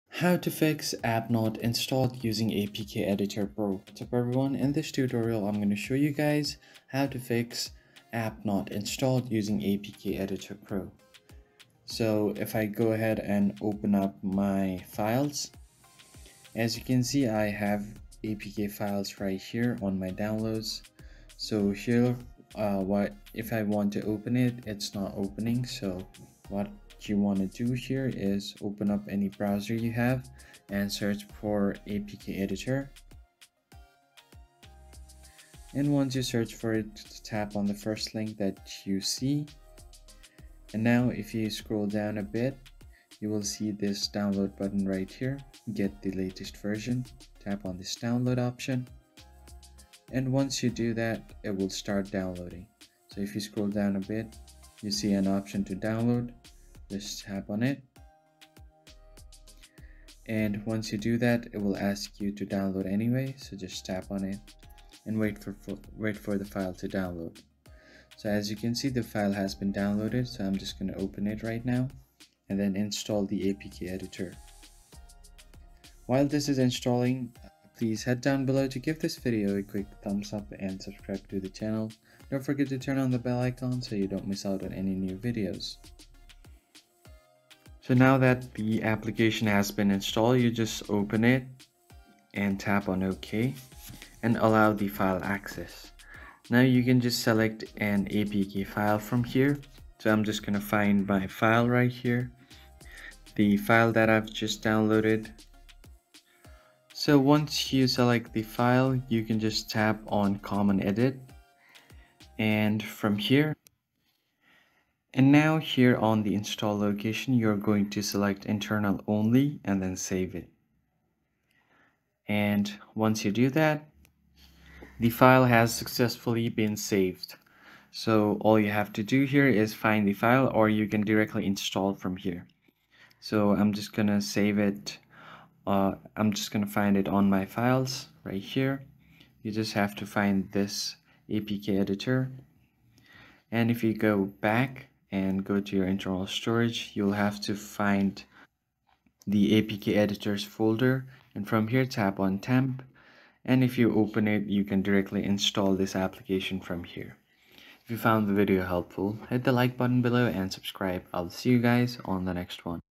How to Fix App Not Installed Using APK Editor Pro
Learn how to fix the "App Not Installed" error on your Android device using APK Editor Pro in this step-by-step guide. Whether you're encountering installation issues or just want to learn how to modify APK files, this tutorial has got you covered!

In this video, you'll discover how to fix the "App Not Installed" error with APK Editor Pro, and we'll guide you through the process of using the app to troubleshoot installation issues and edit APK files.

Timestamps:
0:00 Introduction
0:20 Step 1: Downloading APK Editor Pro
0:45 Step 2: Using APK Editor Pro to Fix Installation Errors
1:15 Step 3: Running APK Editor Pro and Testing the Fix